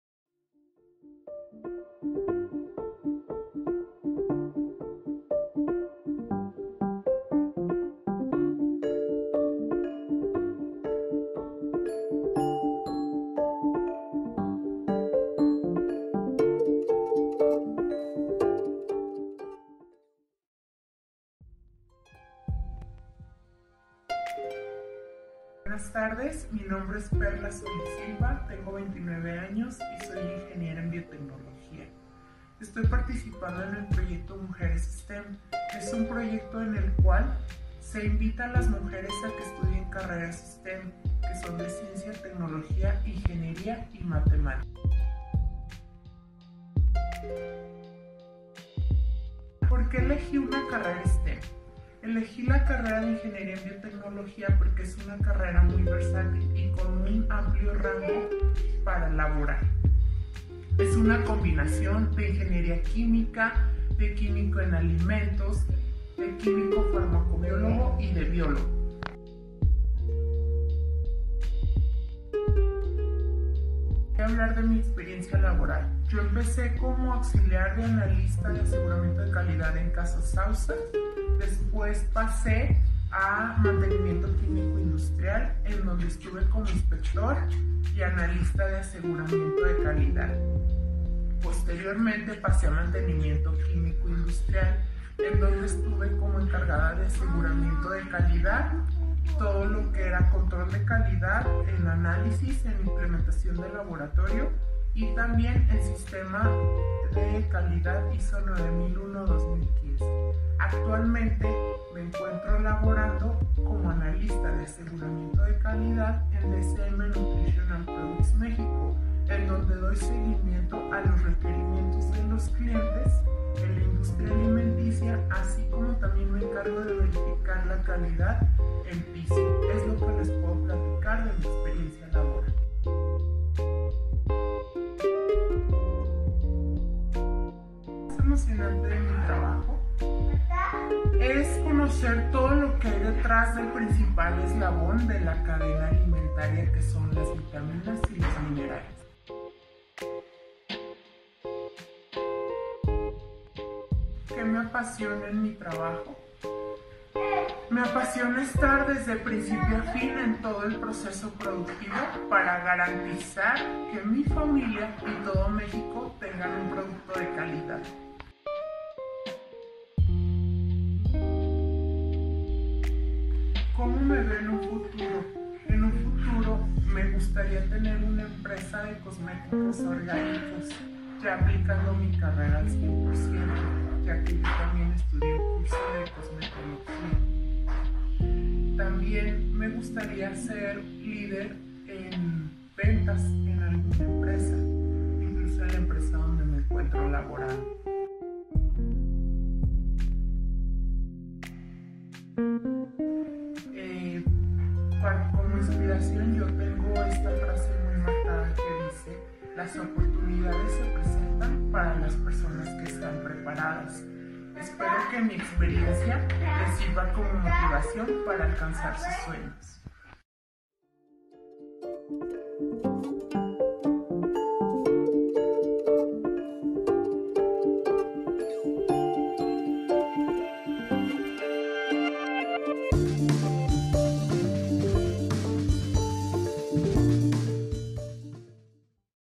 Mujeres W-STEM: Perla Solíz Silva
Perla Solíz Silva es mexicana e ingeniera en biotecnología. Trabaja como analista de aseguramiento de calidad, en proyectos para la industria alimenticia.

Contenido elaborado por Universidad de Guadalajara UG como parte del proyecto W-STEM.

Sutítulos en Inglés realizados por Laura Martínez Urtiaga y Soraya Aguayo Román.